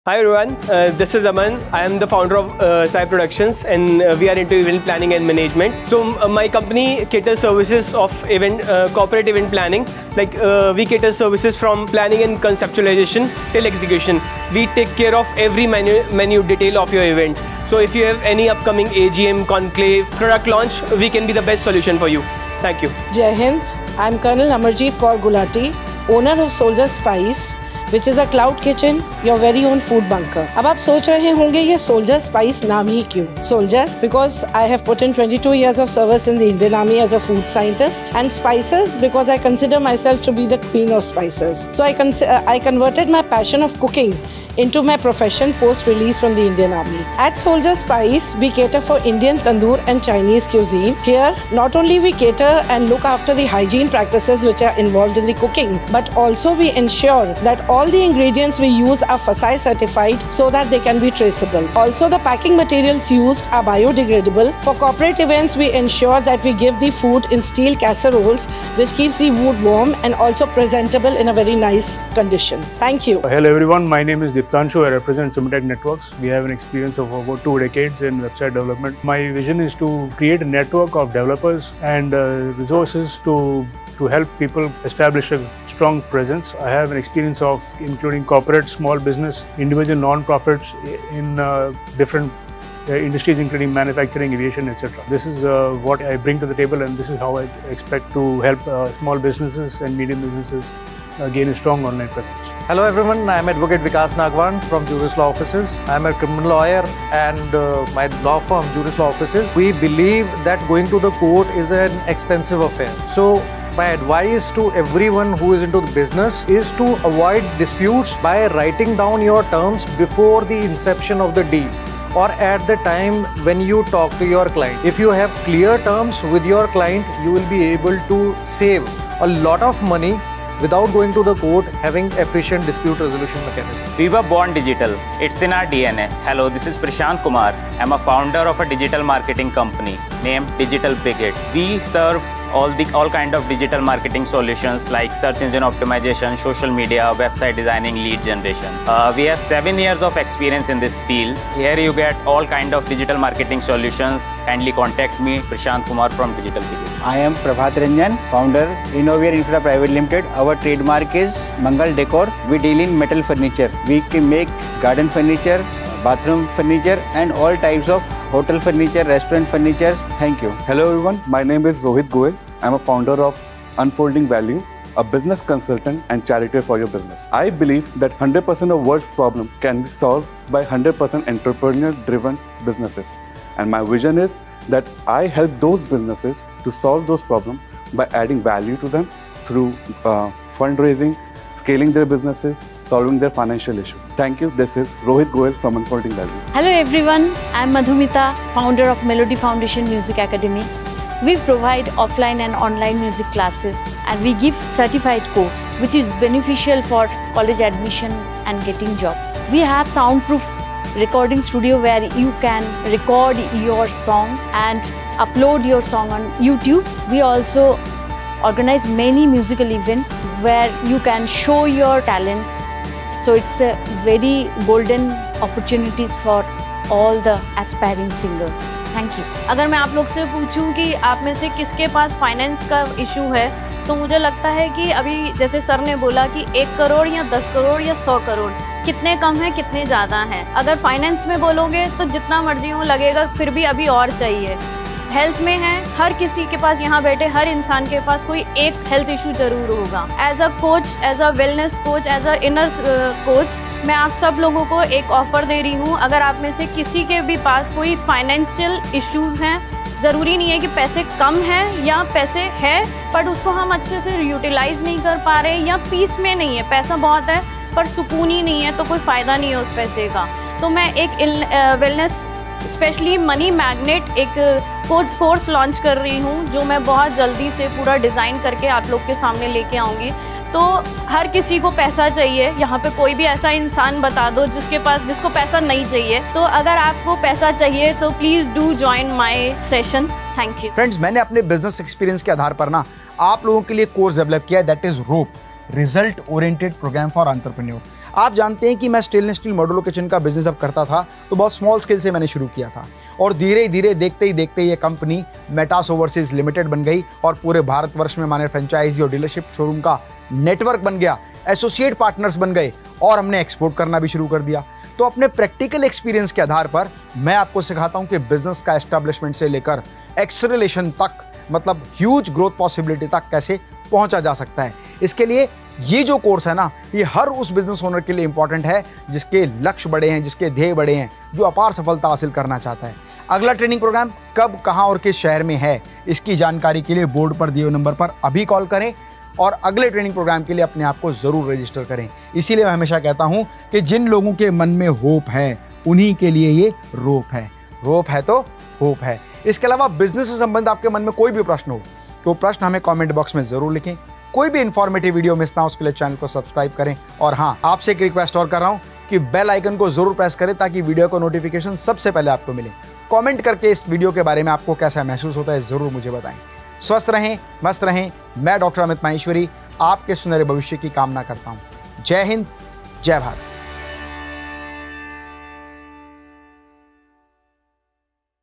Business Introduction of R.O.P.E. Participants Part 2 | Business Workshop By Dr. Amit Maheshwari
📣🎥 R.O.P.E. Business Workshop: Introducing Our Amazing Participants! 🎥📣

In this video, we are thrilled to share highlights from our Result Oriented Program for Entrepreneurs (R.O.P.E.) Business Workshop, where participants from diverse backgrounds gathered to learn, grow, and achieve entrepreneurial success!

🎯 Dr. Amit Maheshwari - A renowned entrepreneur, motivational speaker, and business strategist, empowering participants with his expertise and knowledge.

In this video, you will witness each participant delivering their captivating business introduction, sharing their aspirations, goals, and unique journeys as entrepreneurs. From exciting startups to established ventures, these passionate individuals exemplify the true spirit of entrepreneurship.

The R.O.P.E. Business Workshop provided a platform for networking, learning from industry experts, and gaining valuable insights into scaling their businesses effectively.

🌟 Don't miss this opportunity to witness the enthusiasm and drive of these exceptional entrepreneurs! 🌟